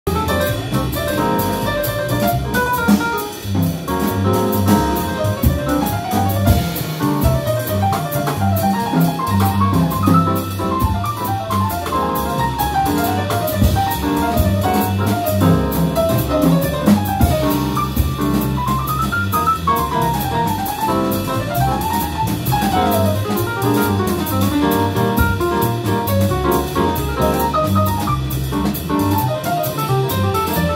Emanuele Rizzo Giuseppe civiletti Giampaolo scatozza live al Elegance Cafè Jazz Club di Roma
Video da parte di Giampaolo Scatozza